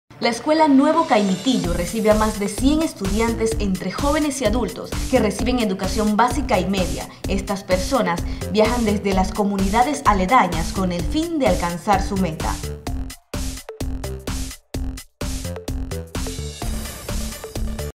Meduca Panama -Tele Educación Capsula 3 - Esc Nuevo Caimitillo
La Escuela Nuevo Caimitillo recibe a mas de 100 estudiantes entre jóvenes y adultos que reciben educación básica y media.